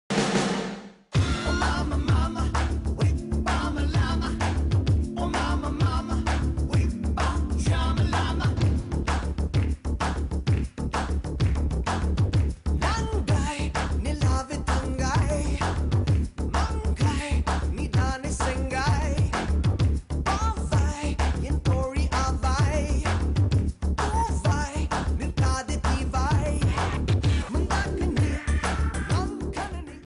Quarter Finals Round 1 - House of Tamil
House of Tamil's performance in Round 1 of the Dhool V Quarter Finals!